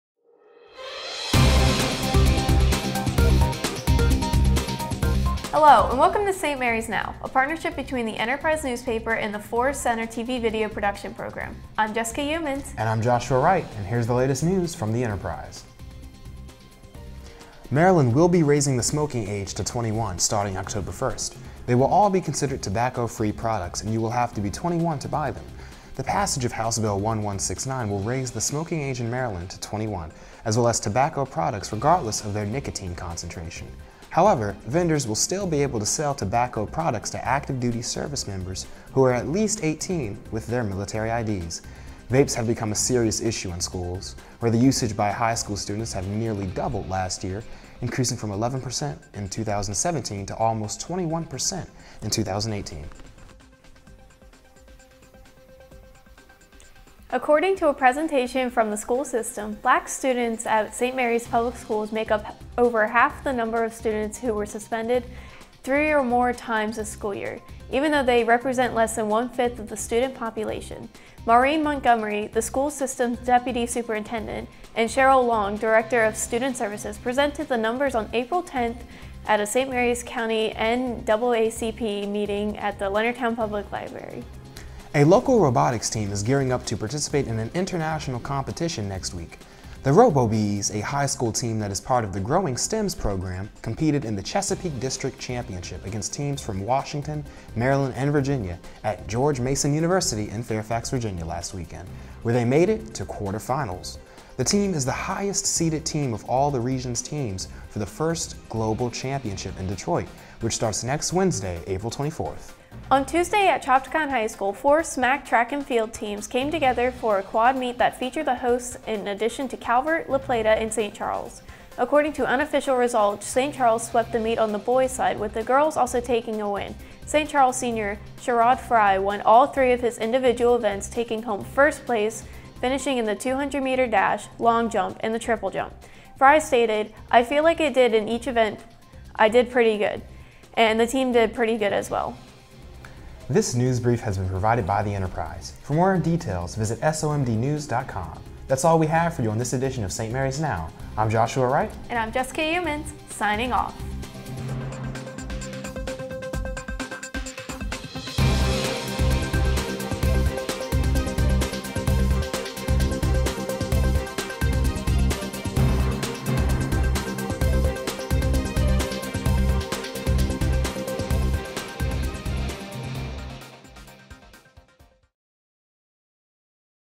St. Mary's Now (from 4-19-2019)
Student produced video archived from the former Bulldog TV Channel.

St. Mary's Now is a partnership between The Enterprise newspaper and the FCTC TV/Video Production program.  This newsbrief is produced by Gold Team and covers stories from The Enterprise's Friday, April 19, 2019 edition.  Stories include: Maryland to raise smoking age to 21, racial disparities in school suspensions, Robobees continue on in competition, and a look at local sports.  Josh and Jessica are your anchors.  Erik is your director and editor.